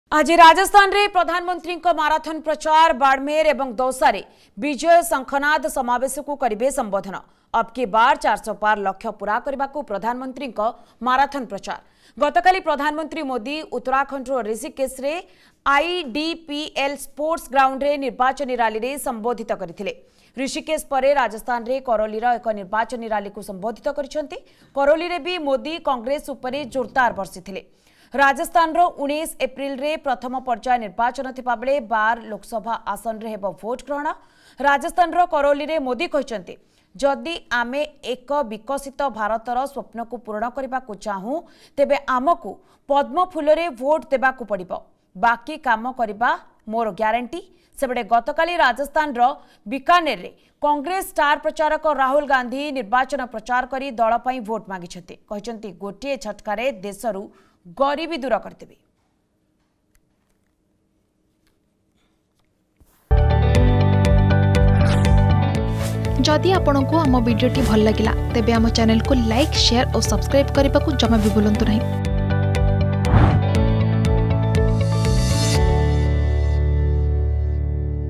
ଆଜି ରାଜସ୍ଥାନରେ ପ୍ରଧାନମନ୍ତ୍ରୀଙ୍କ ମାରାଥନ ପ୍ରଚାର || Narendra Modi
ଆଜି ରାଜସ୍ଥାନରେ ପ୍ରଧାନମନ୍ତ୍ରୀଙ୍କ ମାରାଥନ ପ୍ରଚାର ।